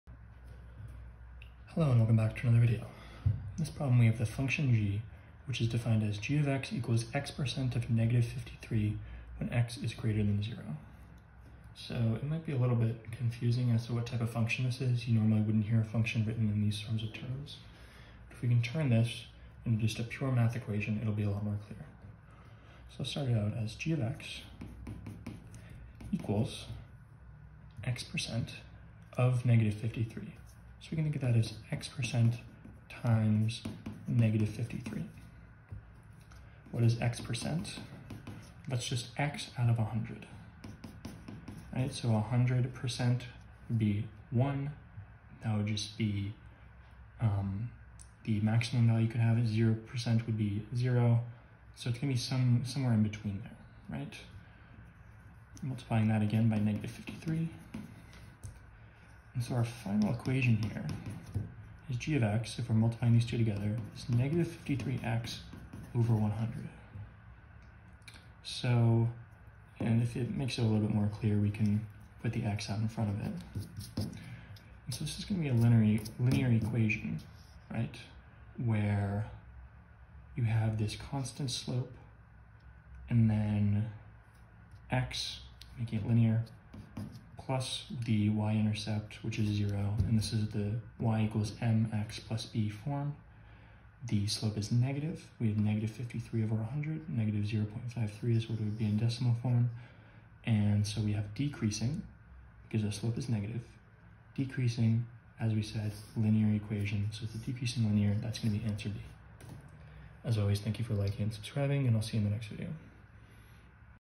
21. The function g is defined as g(x) equals x% of -53, when x⋗0. Which of the following describes
21. The function g is defined as g(x) equals x% of -53, when x⋗0. Which of the following describes this function?

(a) Decreasing exponential
(b) Decreasing linear
(d) Increasing linear

2024 Digital SAT
Test 4 Module 2 Section 1
Problem 21